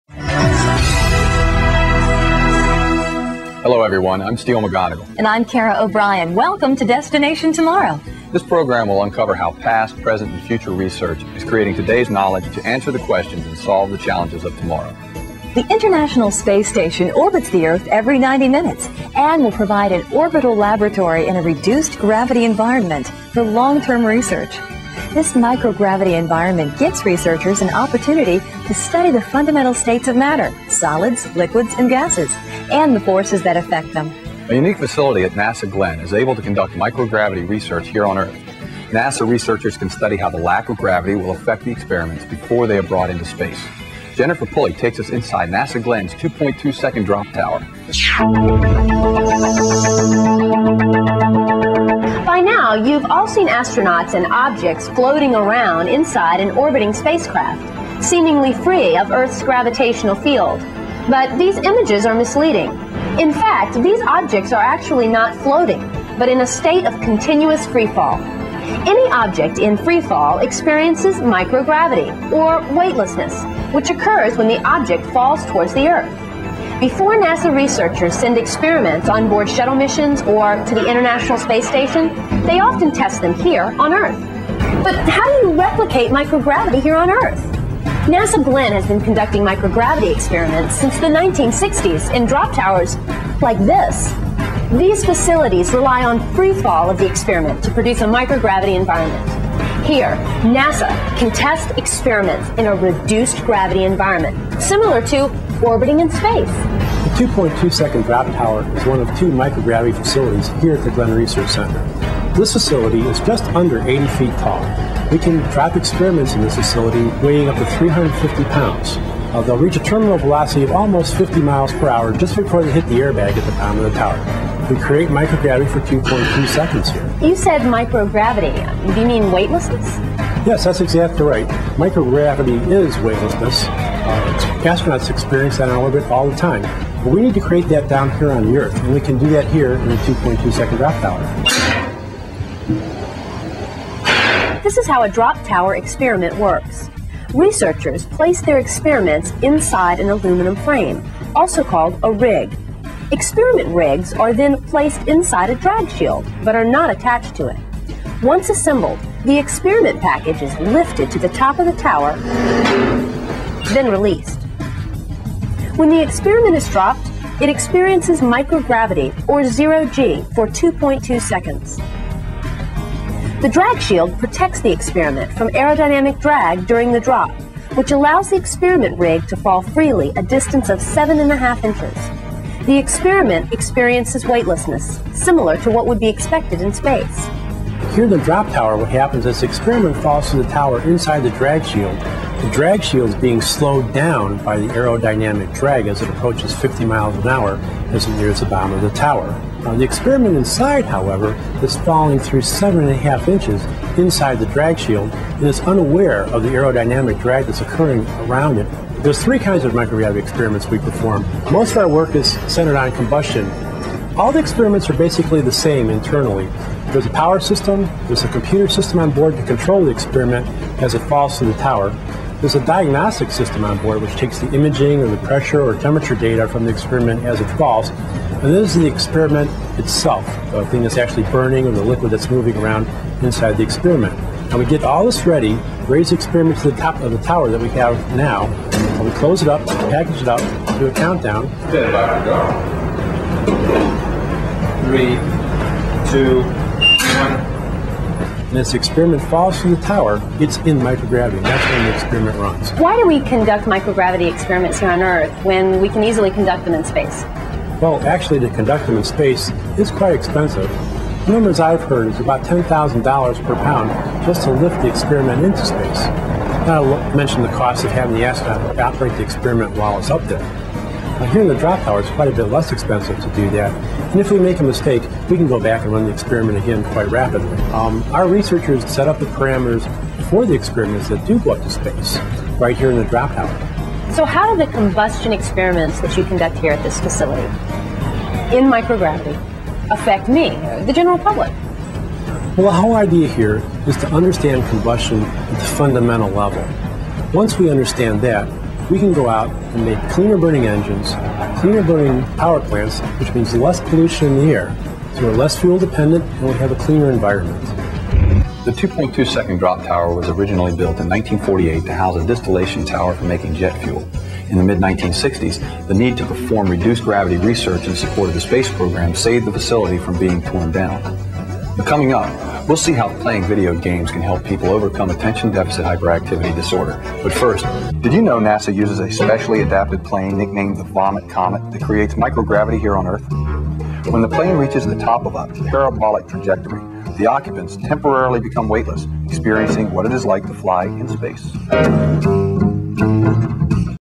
Behind the scenes at the NASA 2.2 Second Drop Tower for microgravity research
This video is a Destination Tomorrow show giving a behind-the-scenes tour of the 2.2 Second Drop Tower at the NASA Glenn Research Center in Cleveland, Ohio.
This facility is used by high school students to operate their DIME experiments.  Also, some middle school experiments will be dropped in this facility as part of DIME 2010.